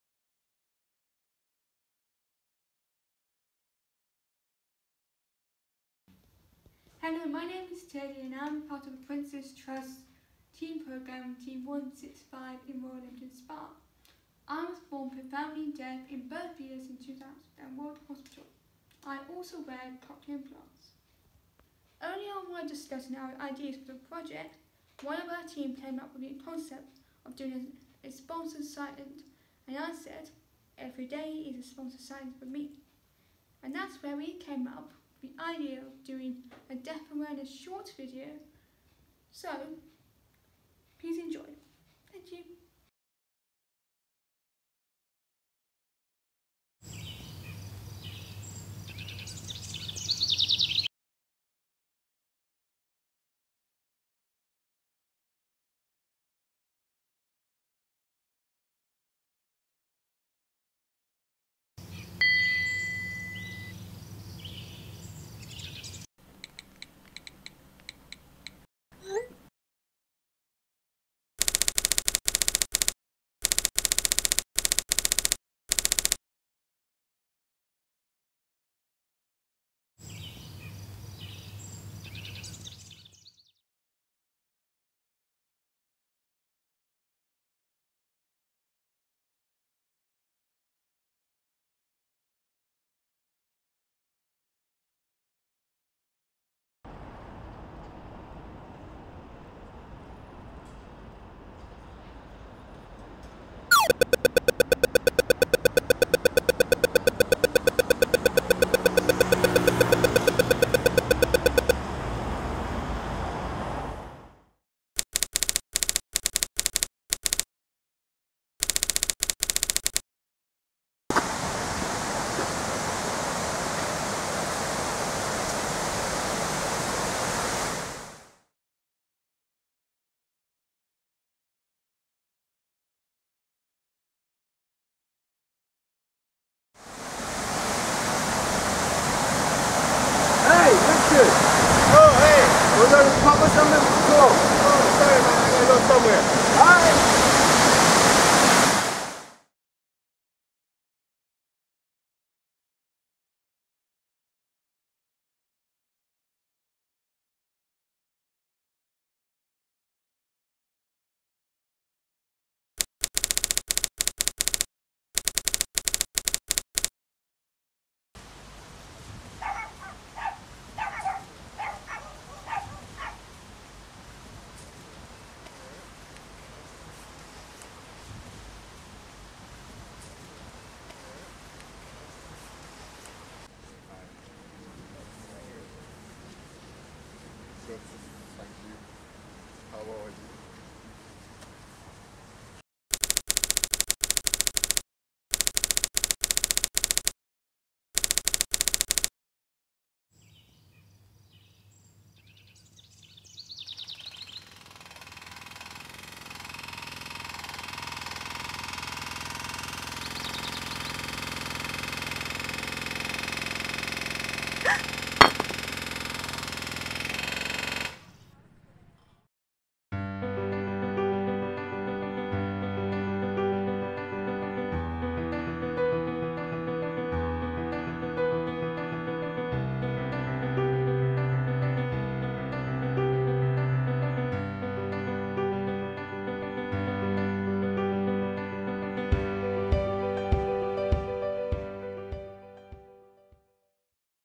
2 Ways 2 Meet - Princes Trust WCG
A short deaf awareness film created by Leamington Spa Prince's Trust Team 165 (group A)